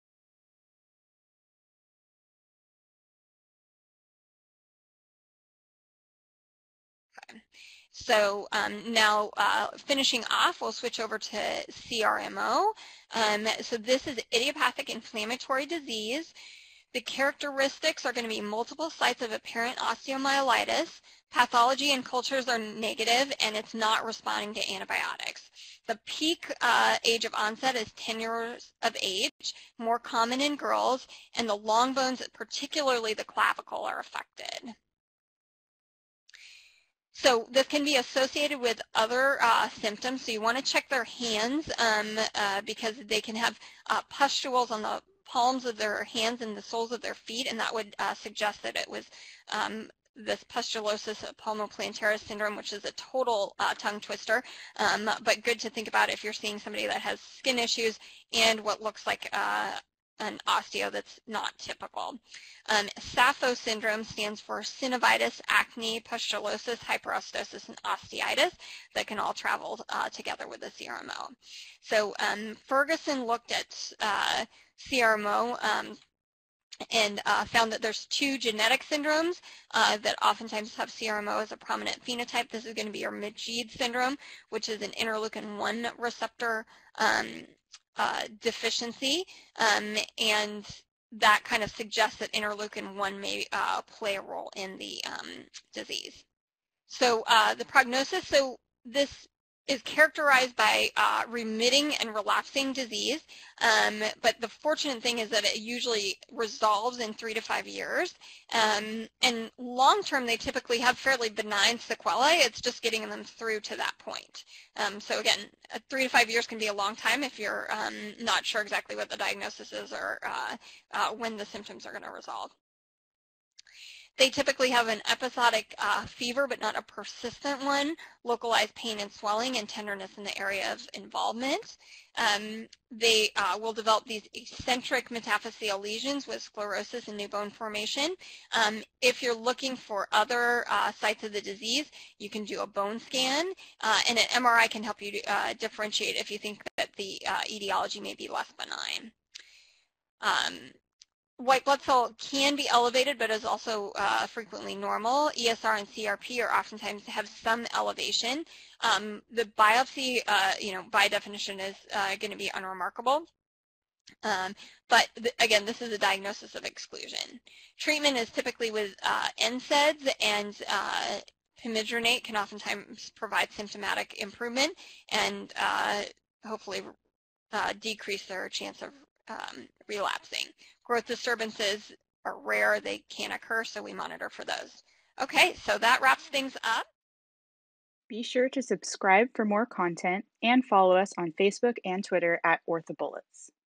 Chronic Recurrent Mulitfocal Osteomyelitis (CRMO) Exam Review - Lindsay Andras, MD
Chronic Recurrent Mulitfocal Osteomyelitis (CRMO) Exam Review
From: Pediatric Orthopaedic Infections Core Curriculum Webinar

Lindsay Andras, MD
Attending Physician
Assistant Professor of Clinical Surgery, Keck School of Medicine of USC
Children's Hospital of Los Angeles